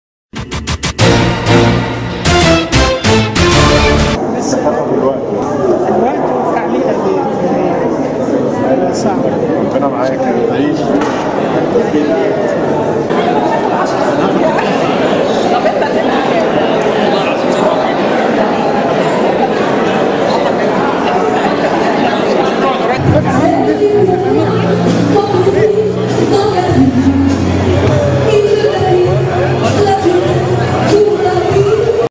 سميح ساويرس يتجاهل الصحفيين‎
سميح ساويرس رفض التعامل مع الصحافة، مساء أمس، فى حفل توقيع كتاب زاهى حواس "40 سنة حفائر".
رجل الأعمال كان حريصاً طوال الوقت على الابتعاد عن عدسات المصورين، وكان يرد على أسئلتهم بابتسامة مغادراً مكانه فوراً ومنتقلاً إلى مكان آخر، يكون أكثر زحاماً حتى لا يستطيع المصورون الوصول إليه بسهولة.